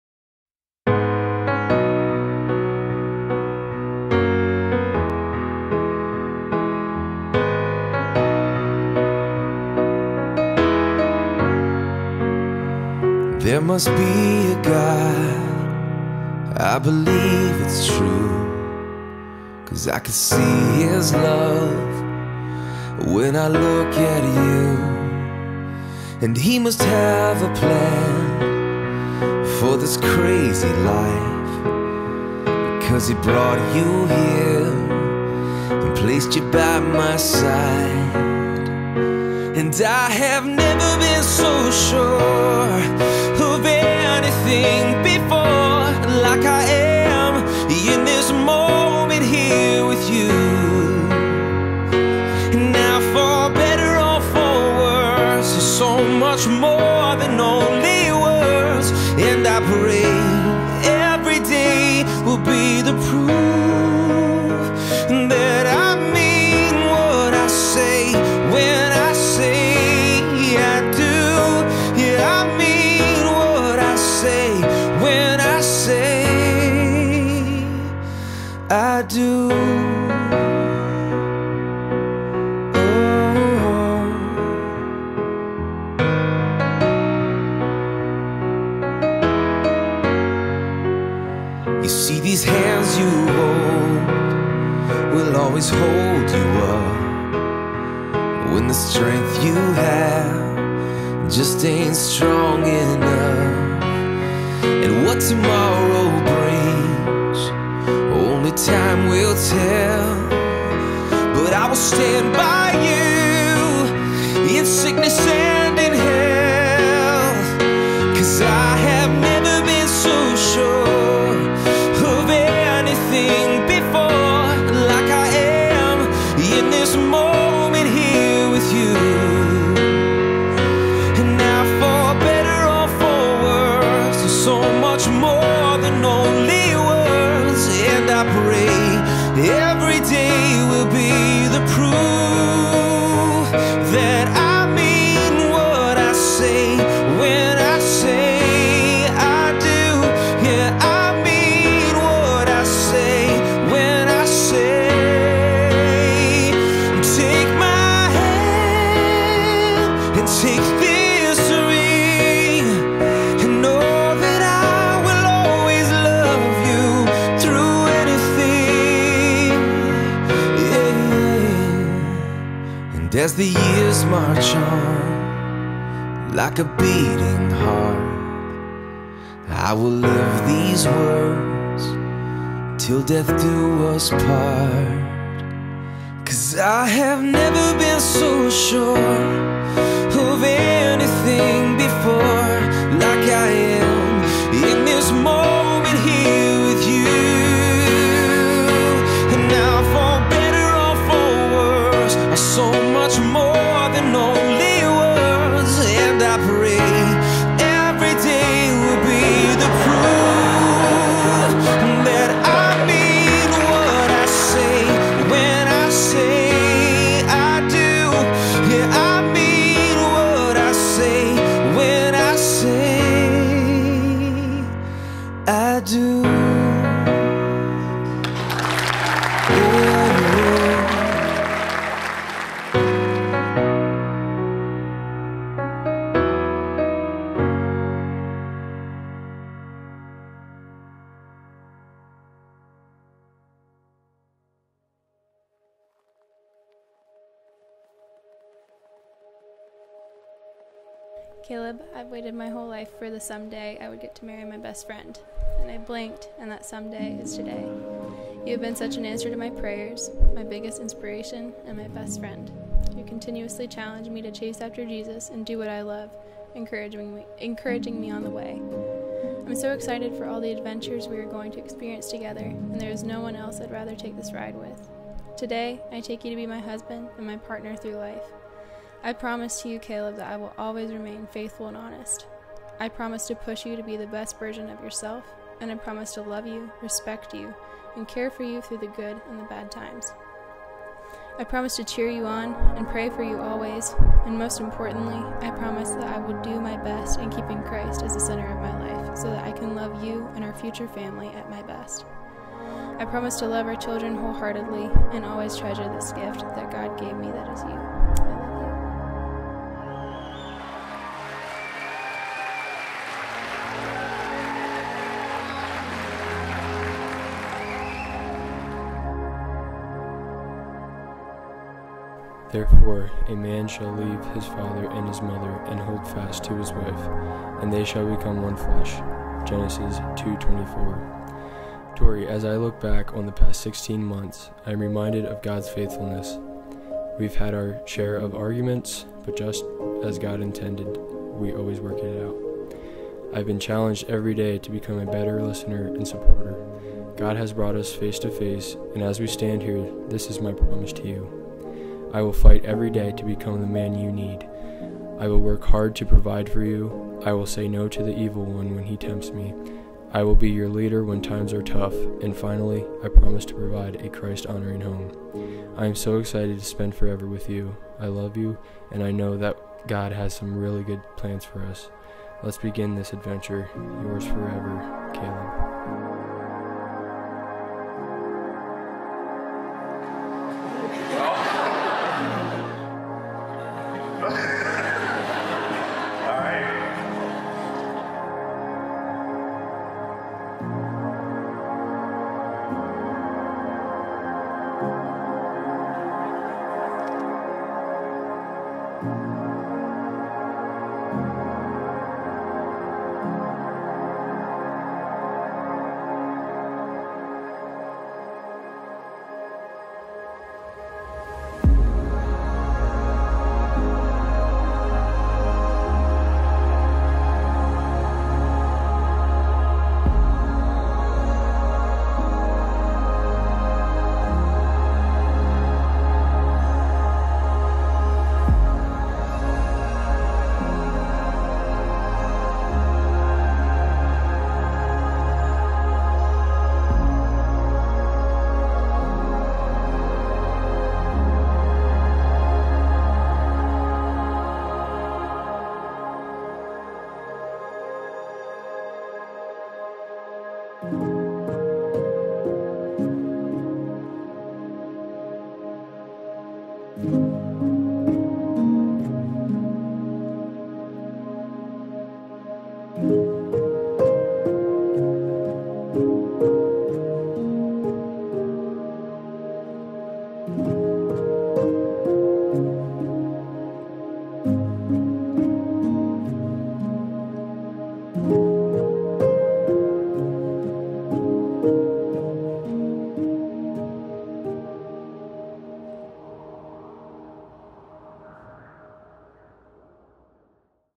Caleb & Tori | 04.30.22
The wedding of Caleb and Tori Miller celebrated on April 30, 2022.

Equipment used -
Camera: Panasonic GH5
Lenses: Tamron 28-70mm/2.8, Sigma 18-35/1.8
Recorder: Zoom H6
Monitor: Atomos Ninja V